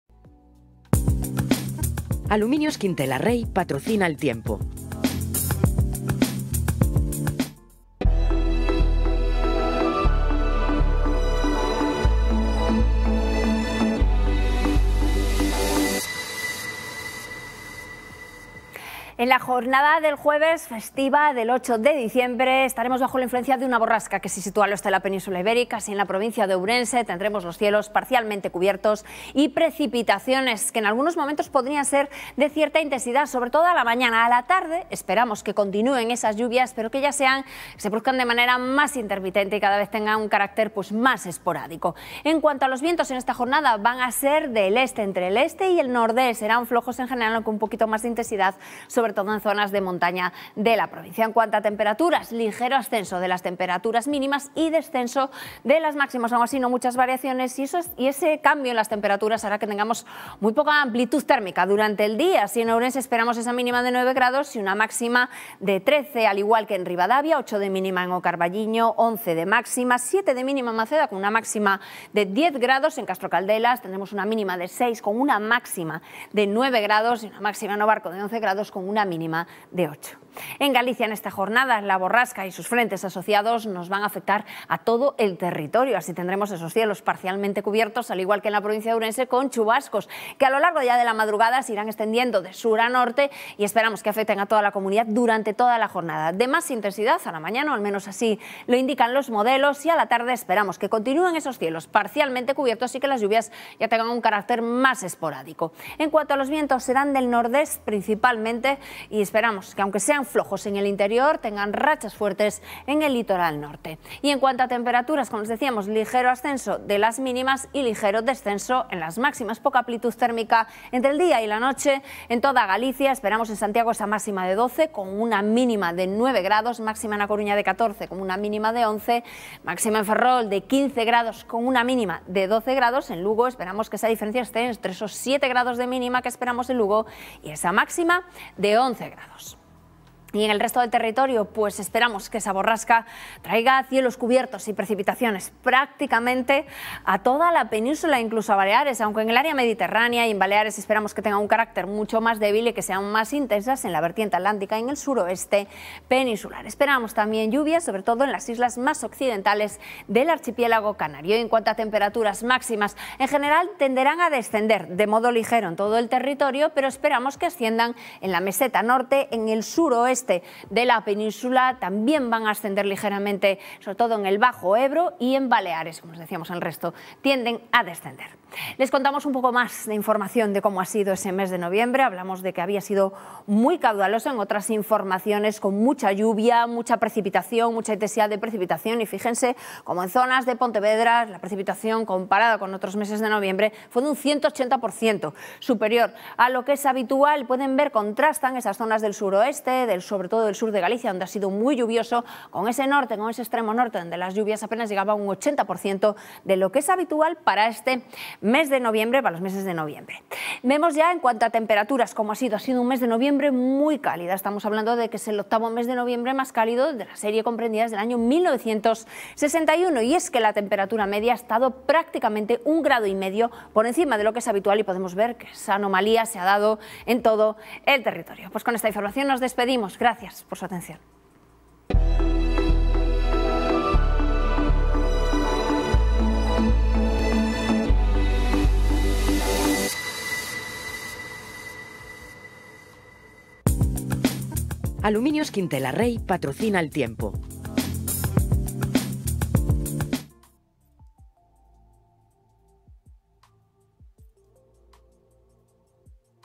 ¿Qué tiempo hará este jueves 8 de diciembre en Ourense?
La predicción del tiempo para este jueves festivo en Ourense